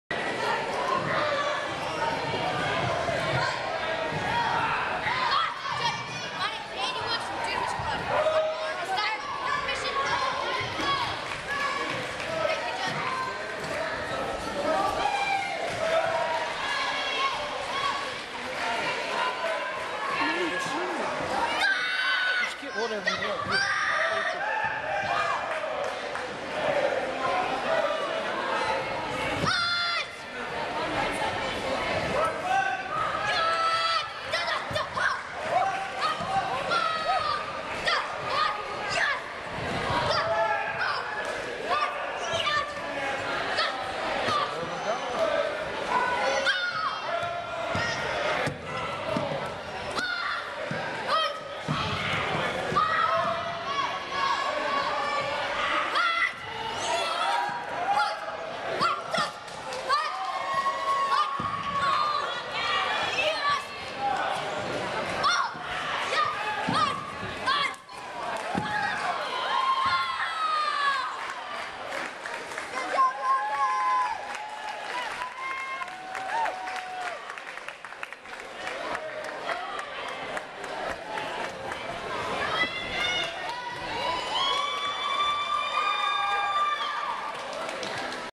Andi Woods - Open Kata from Goodwill Tournament 2011
Andi Woods - 12-13 year old Black Belt Division - Goodwill Tournament, Lady's Island, SC - March 5, 2011 - Open Kata Division